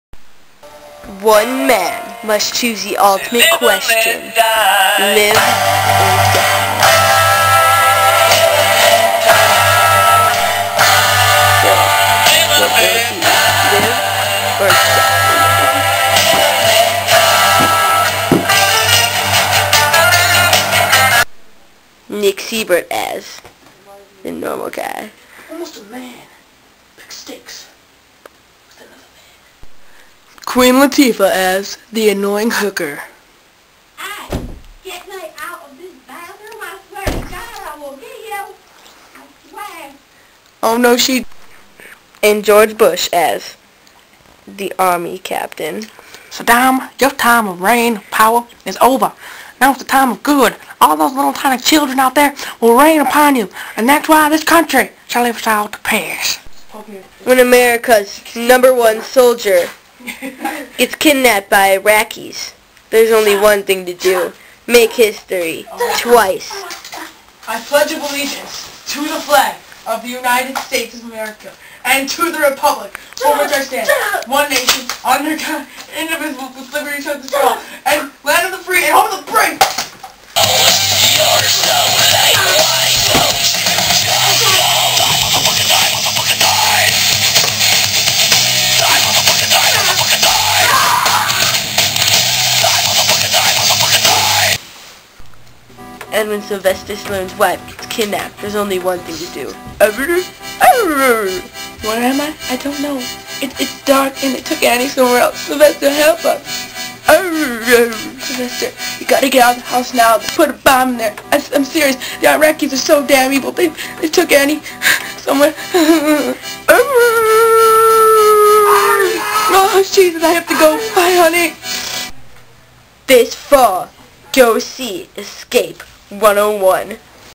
Escape 101
In this epic trailor for the new movie Escape 101 you get a sneak peak of the film.
Camera and Queen Latifa and Sylvestor Stallone-Zach
Guy, George W. Bush,And Camera and Wife-Nic
Terrorists-Josh and Ed